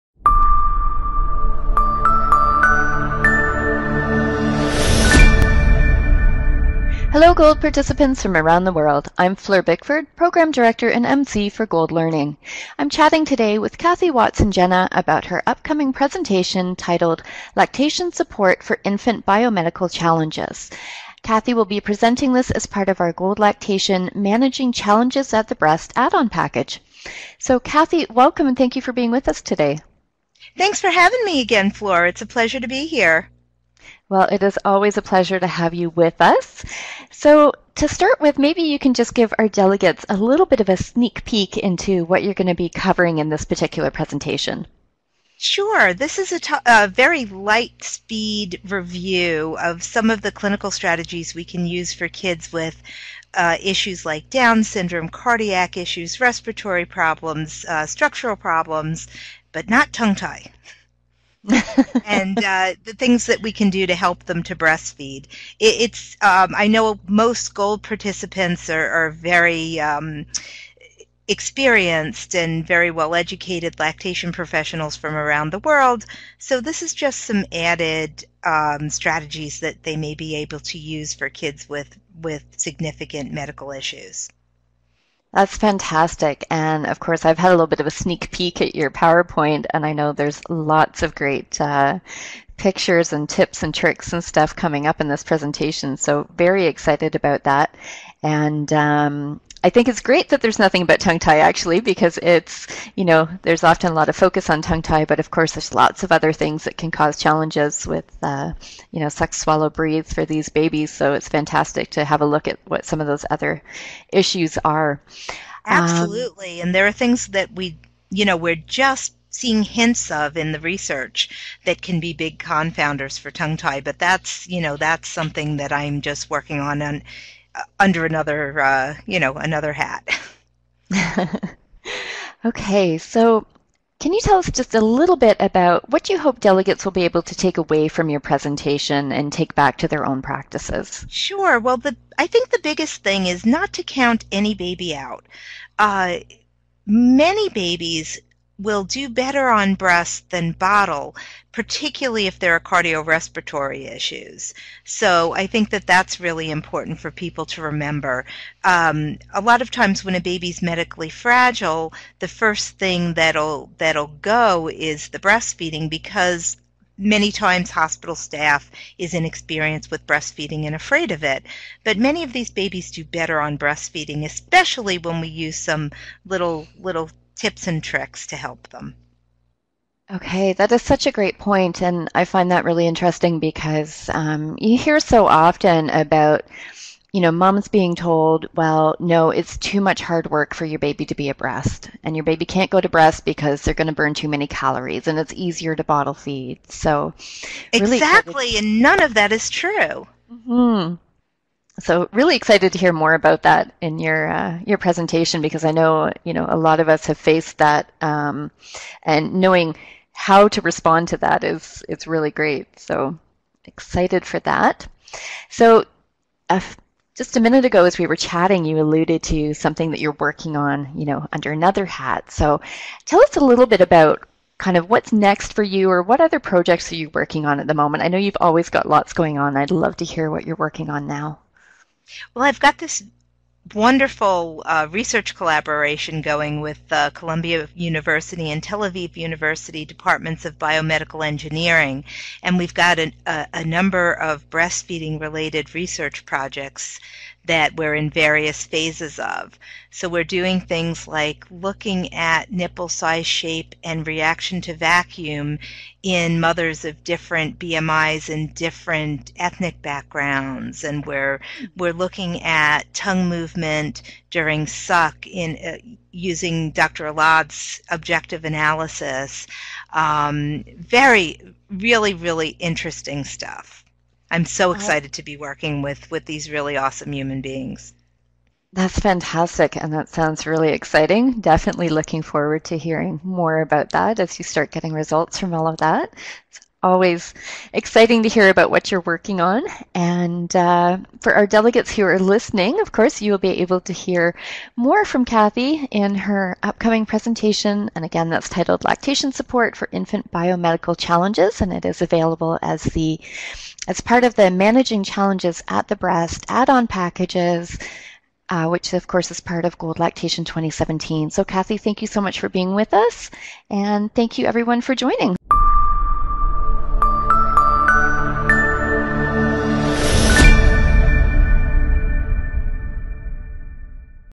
Interview with Catherine Watson Genna for our Challenges in Breastfeeding Add-on Lecture Pack 2017
We're thrilled to have Catherine Watson Genna, BS, IBCLC, presenting: "Lactation Support for Infant Biomedical Challenges", as a part of our GOLD Lactation Milk Banking Add-on Lectures.